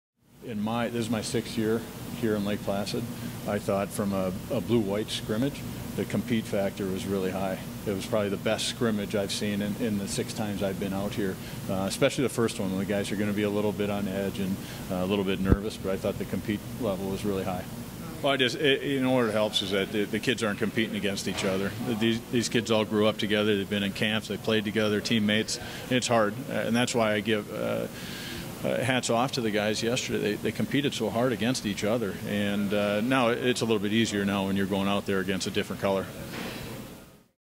Mark Osiecki Reviews First Day of U.S. NJEC - 2014 USA Hockey
Head Coach Mark Osiecki discusses the USA Blue/White Scrimmage and talks about the first international games against Sweden and Finland at the 2014 U.S. National Junior Evaluation Camp..